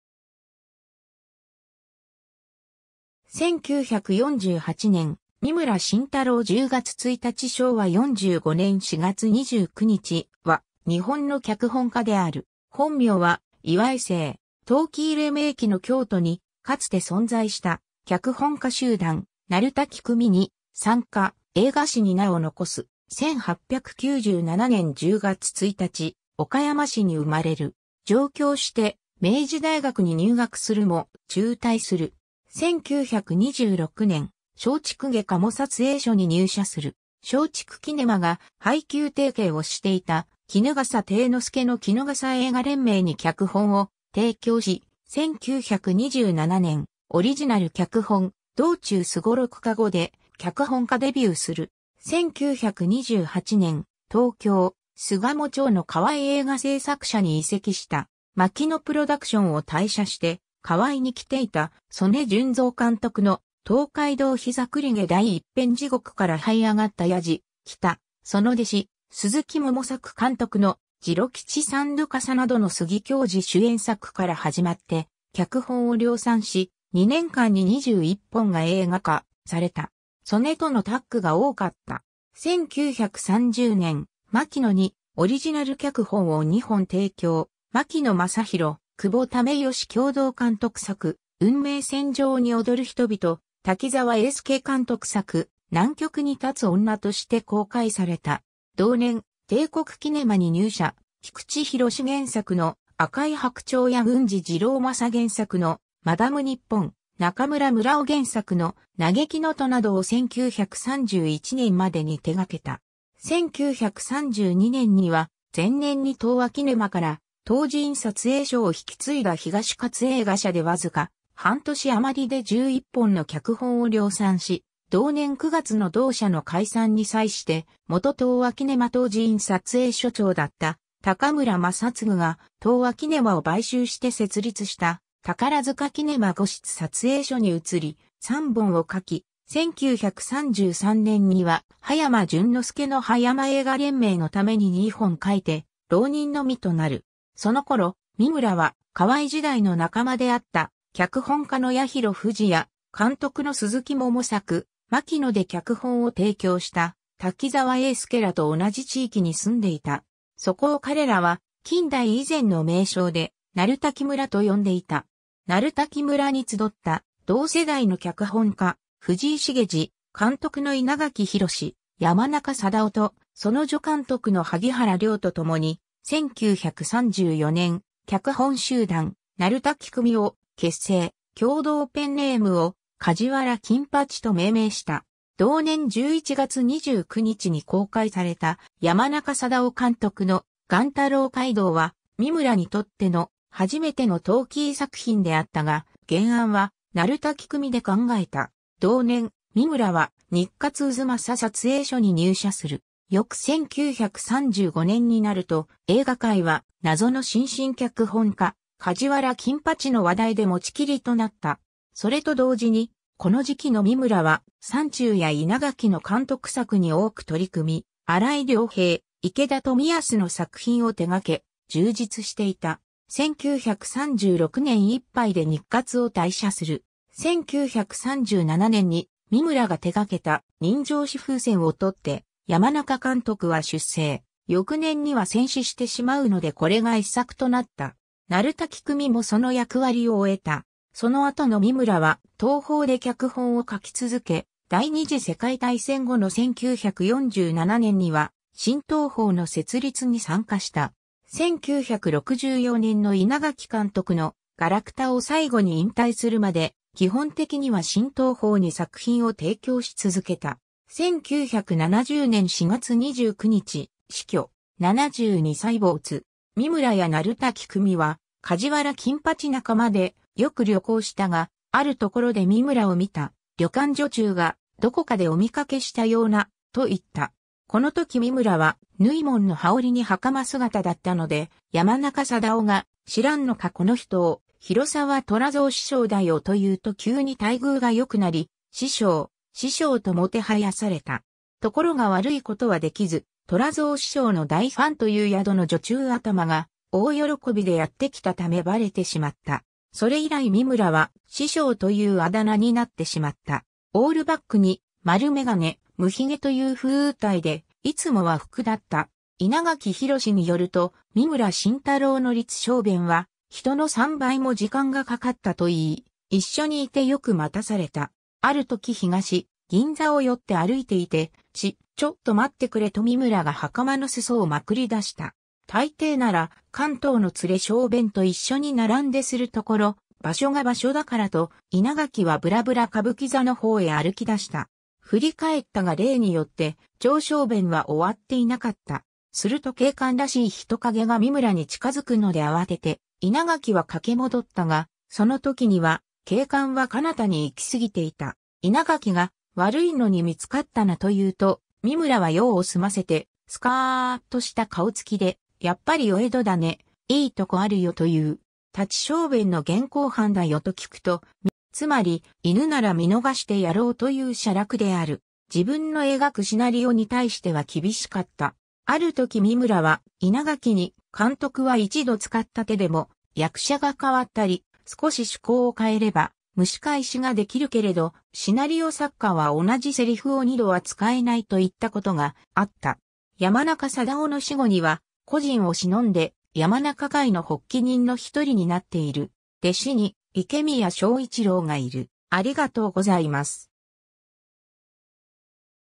三村伸太郎
1948年 三村 伸太郎（みむら しんたろう、明治30年（1897年）10月1日 - 昭和45年（1970年）4月29日）は、日本の脚本家である。
本名は岩井（いわい）姓。
トーキー黎明期の京都にかつて存在した脚本家集団「鳴滝組」に参加、映画史に名を残す。
 1897年（明治30年）10月1日、岡山市に生まれる。
上京して、明治大学に入学するも、中退する。
 1926年（大正15年）、松竹下加茂撮影所に入社する。
松竹キネマが配給提携をしていた衣笠貞之助の衣笠映画連盟に脚本を提供し、1927年（昭和2年）、オリジナル脚本『道中双六駕籠』で脚本家デビューする。
1928年（昭和3年）、東京・巣鴨町（現在の豊島区西巣鴨）の河合映画製作社に移籍した。
マキノ・プロダクションを退社して河合に来ていた曾根純三監督の『東海道膝栗毛 第一篇 地獄から這上った弥次喜多』、その弟子・鈴木桃作監督の『次郎吉三度笠』などの杉狂児主演作からはじまって、脚本を量産し、2年間に21本が映画化された。
曾根とのタッグが多かった。
 1930年（昭和5年）、マキノにオリジナル脚本を2本提供、マキノ正博・久保為義共同監督作『運命線上に躍る人々』、滝沢英輔監督作『南極に立つ女』として公開された。
同年、帝国キネマに入社、菊池寛原作の『赤い白鳥』や郡司次郎正原作の『マダムニッポン』、中村武羅夫原作の『嘆きの都』などを1931年（昭和6年）までに手がけた。
 1932年（昭和7年）には、前年に東亜キネマから等持院撮影所を引き継いだ東活映画社でわずか半年あまりで11本の脚本を量産し、同年9月の同社の解散に際して、元東亜キネマ等持院撮影所長だった高村正次が東亜キネマを買収して設立した宝塚キネマ御室撮影所に移り、3本を書き、1933年（昭和3年）には葉山純之輔の葉山映画連盟のために2本書いて、浪人の身となる。
 そのころ、三村は、河合時代の仲間であった脚本家の八尋不二や監督の鈴木桃作、マキノで脚本を提供した滝沢英輔らと同じ地域に住んでいた。
そこを彼らは近代以前の名称で「鳴滝村」（京都市右京区鳴滝音戸山町）と呼んでいた。
「鳴滝村」に集った同世代の脚本家・藤井滋司、監督の稲垣浩、山中貞雄とその助監督の萩原遼とともに、1934年（昭和9年）、脚本集団「鳴滝組」を結成、共同ペンネームを「梶原金八」と命名した。
同年11月29日に公開された山中貞夫監督の『雁太郎街道』は、三村にとっての初めてのトーキー作品であったが、原案は「鳴滝組」で考えた（「梶原金六」名義）。
同年、三村は日活太秦撮影所に入社する。
 翌1935年（昭和10年）になると、映画界は、謎の新進脚本家「梶原金八」の話題で持ちきりとなった。
それと同時にこの時期の三村は、山中や稲垣の監督作に多く取り組み、荒井良平、池田富保の作品を手がけ、充実していた。
1936年（昭和11年）いっぱいで日活を退社する。
1937年（昭和12年）に三村が手がけた『人情紙風船』を撮って山中監督は出征、翌年には戦死してしまうのでこれが遺作となった。
「鳴滝組」もその役割を終えた。
 その後の三村は、東宝で脚本を書き続け、第二次世界大戦後の1947年（昭和22年）には新東宝の設立に参加した。
1964年（昭和39年）の稲垣監督の『がらくた』（東宝）を最後に引退するまで、基本的には新東宝に作品を提供し続けた。
 1970年（昭和45年）4月29日、死去。
72歳没。
 三村や鳴滝組は「梶原金八」仲間でよく旅行したが、あるところで三村を見た旅館女中が「どこかでお見かけしたような・・・」と言った。
このとき三村は縫紋の羽織に袴姿だったので、山中貞雄が「知らんのかこの人を、広沢虎造師匠だよ」と言うと急に待遇が良くなり、「師匠、師匠」ともてはやされた。
ところが悪いことはできず、虎造師匠の大ファンという宿の女中頭が大喜びでやってきたためバレてしまった。
それ以来三村は「師匠」という綽名になってしまった。
 オールバックに丸眼鏡、無髭という風体で、いつも和服だった。
稲垣浩によると「三村伸太郎の立小便は人の三倍も時間がかかった」といい、一緒にいてよく待たされた。
あるとき東銀座を酔って歩いていて、「チ、ちょっと待ってくれ」と三村が袴の裾をまくりだした。
たいていなら「関東のつれ小便」と一緒に並んでするところ、場所が場所だからと稲垣はぶらぶら歌舞伎座の方へ歩き出した。
振り返ったが例によって長小便は...